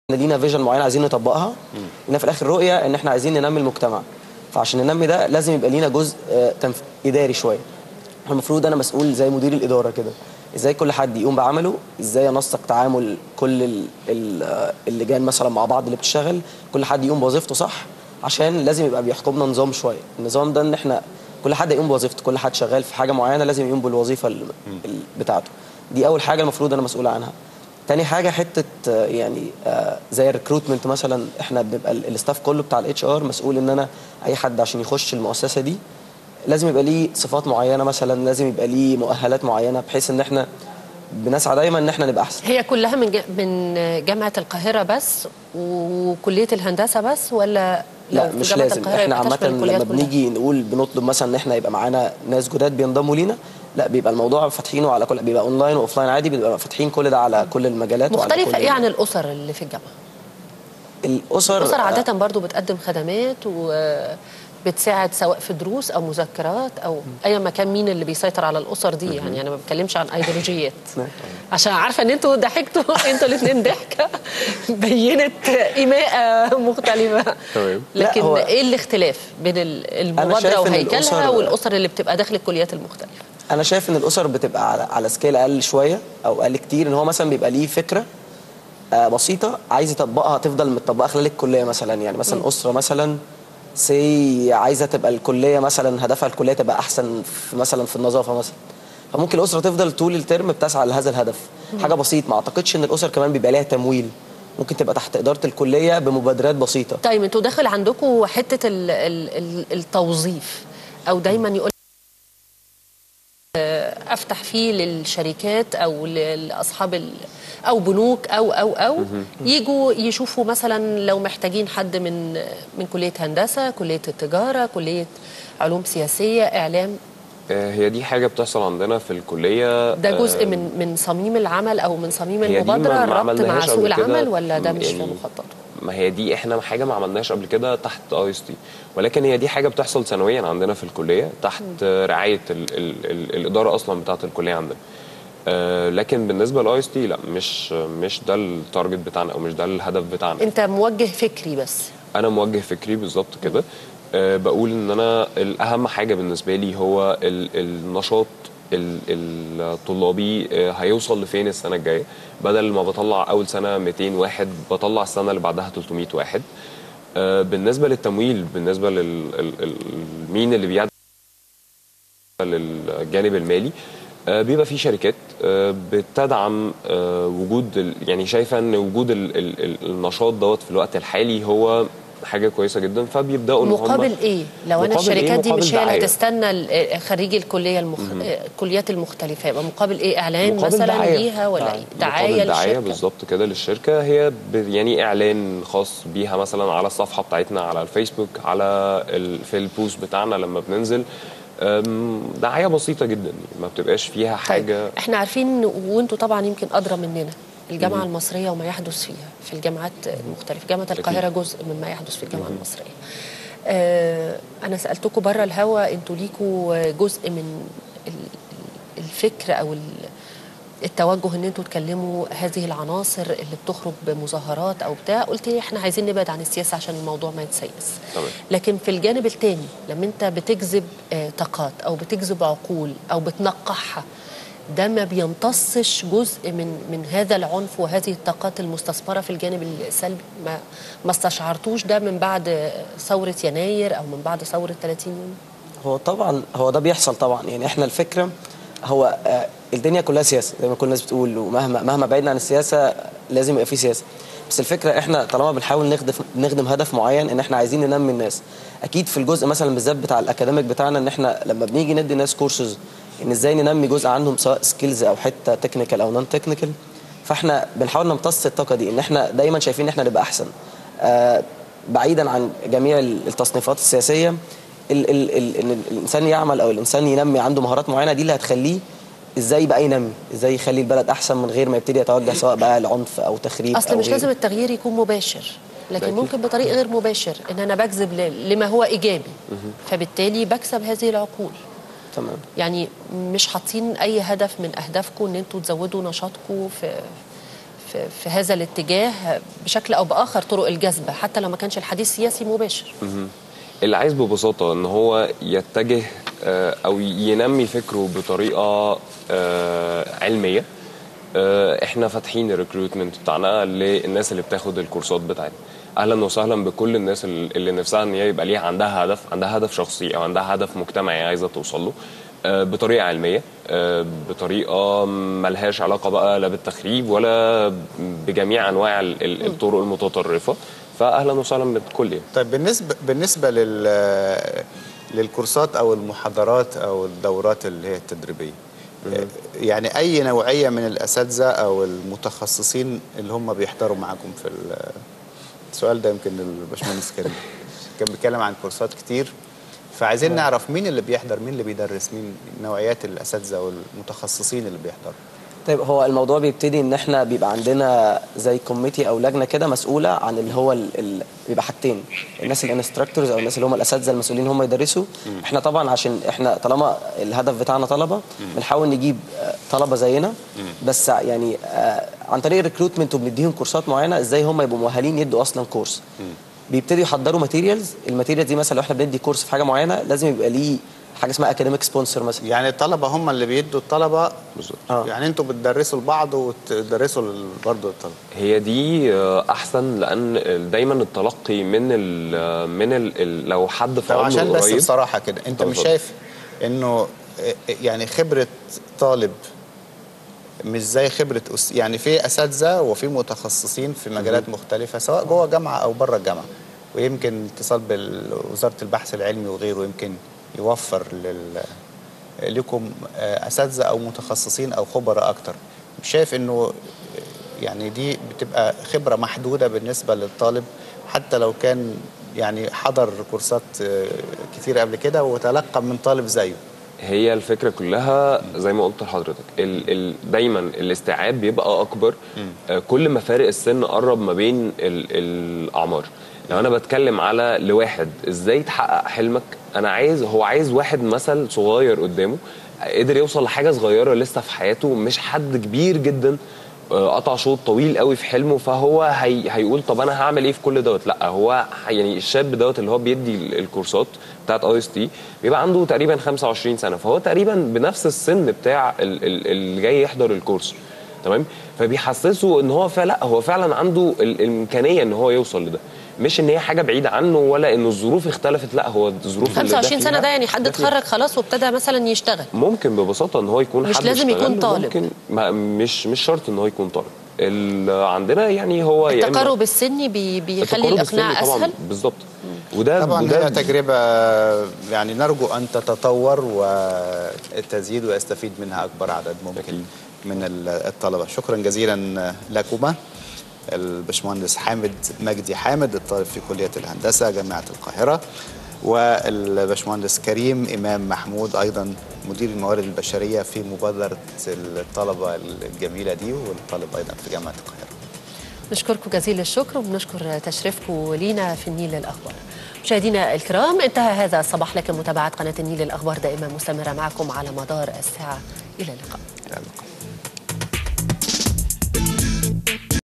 مبادرة لشباب الجامعات تحت عنوان شباب الجامعات الأهداف والأولويات(2)
فقراءت ولقاءات لموضوعات متنوعه صباحاً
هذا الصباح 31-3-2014
قراءة فى صحافة الأثنين
الإعلام بين الواقع والمأمول
مبادرة لشباب الجامعات تحت عنوان شباب الجامعات الأهداف والأولويات